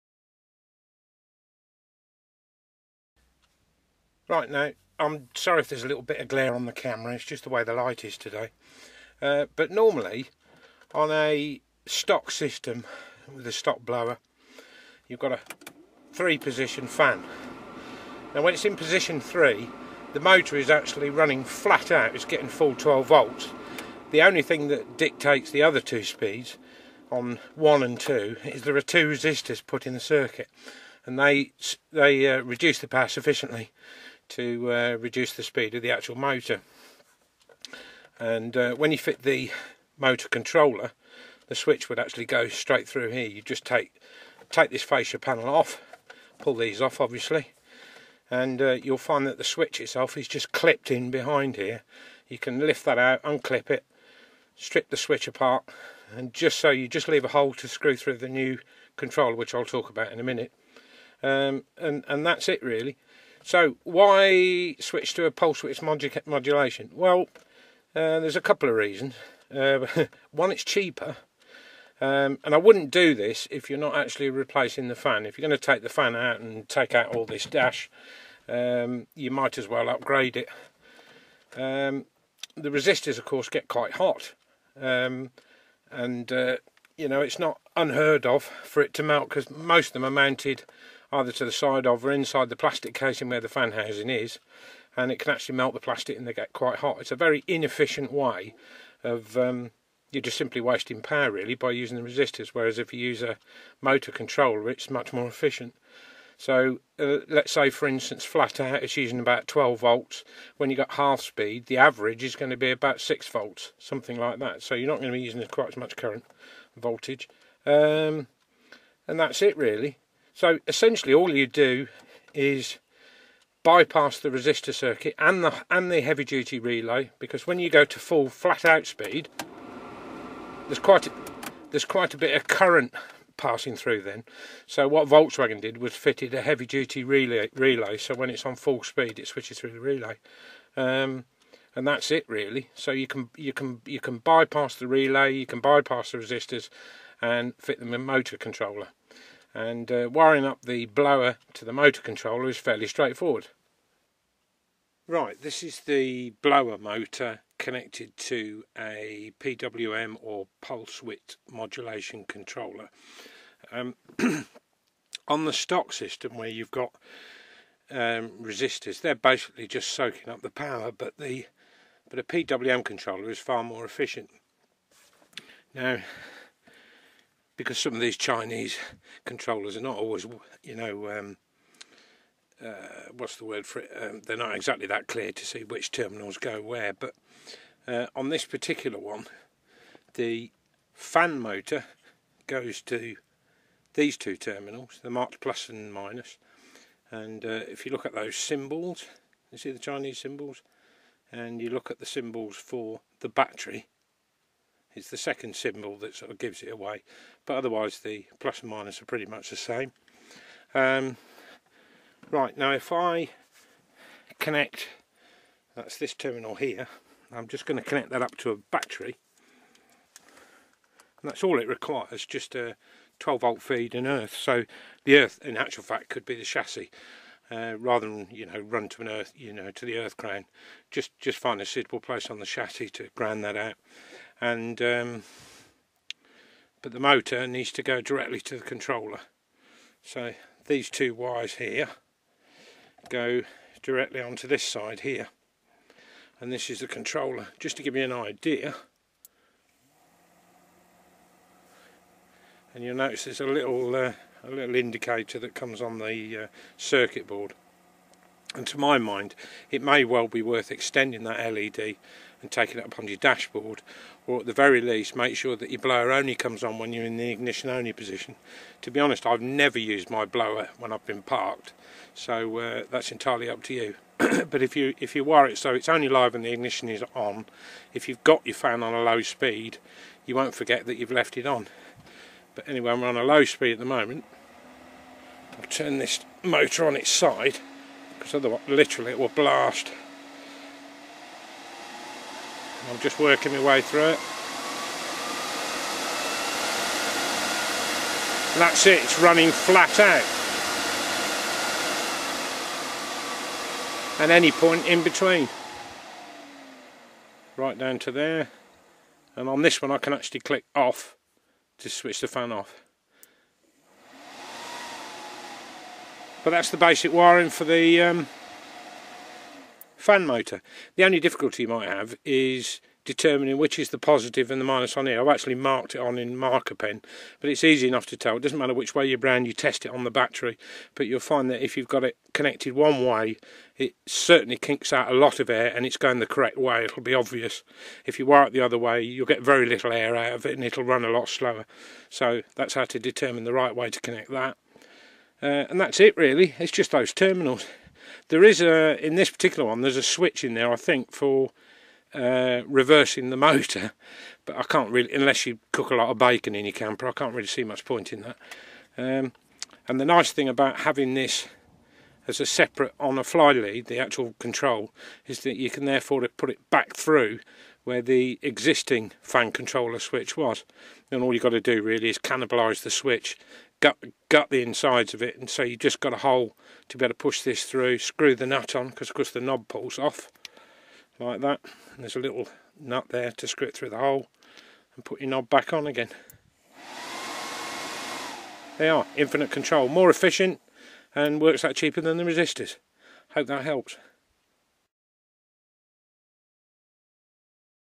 Vanagon VW T25 T3 Blower Motor - PWM control upgrade (PT 1)
How to bypass the resistors entirely and fit a modern PWM motor controller. Controller has to be rated at 20amps minimum and control on flylead makes fitting easier. Controller was just shy of £9 on ebay. The controller should NOT be fitted to an old fan motor as it could exceed rating of controller on startup. The earth lead from motor which normally goes to earth crown needs to be located and moved to negative on fan side of controller instead. Project